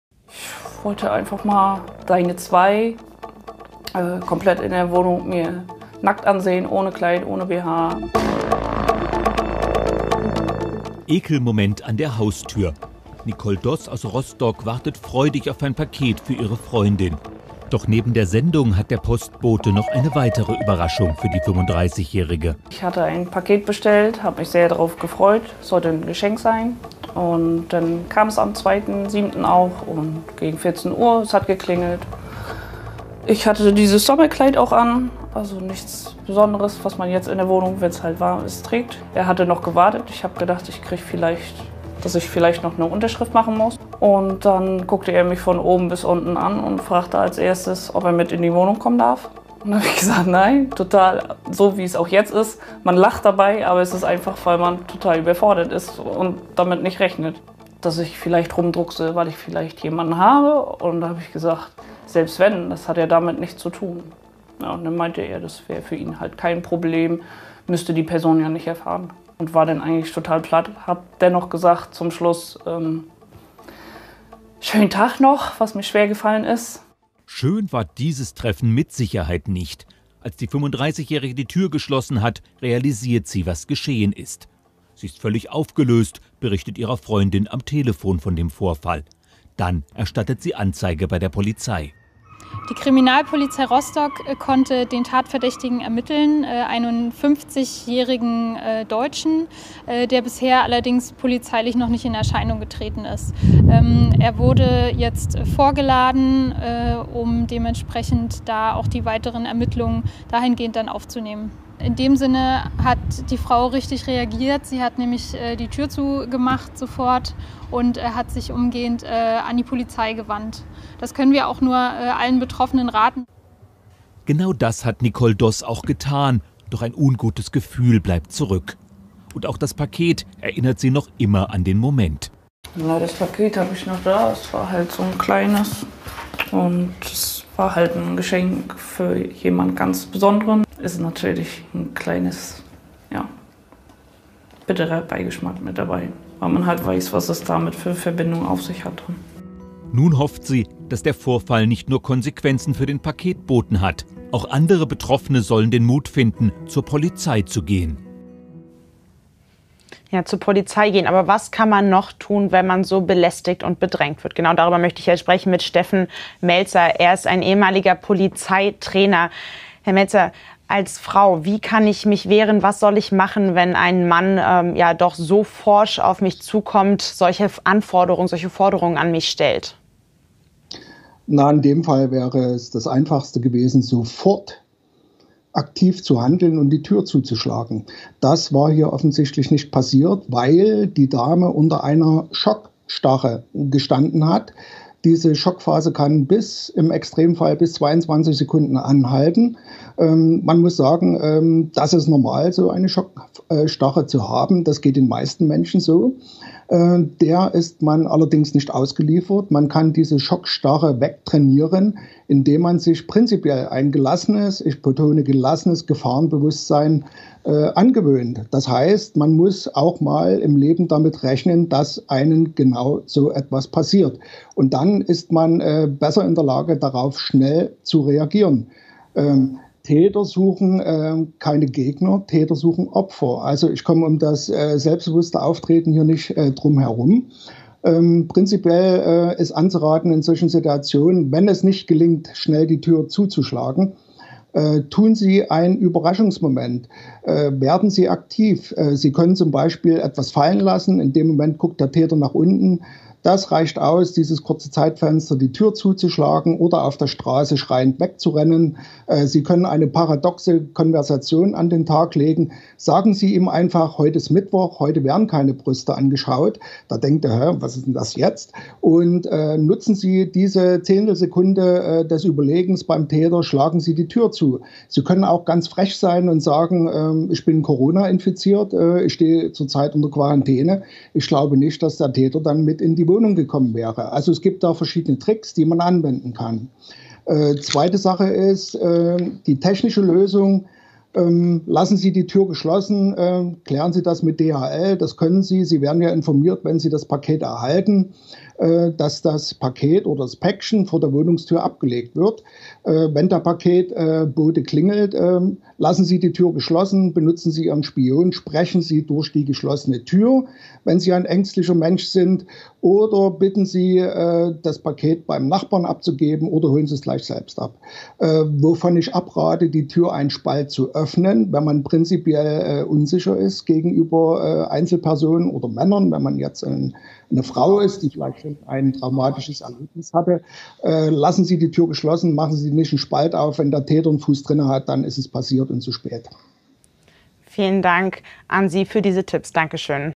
So kannst du dich vor sexueller Belästigung im Alltag schützen
In Rostock wurde eine Frau von ihrem Paketboten sexuell belästigt: Er forderte sie auf, ihm ihre Brüste zu zeigen. Ex-Polizeitrainer Meltzer erklärt, wie du dich vor solchen Übergriffen schützen kannst.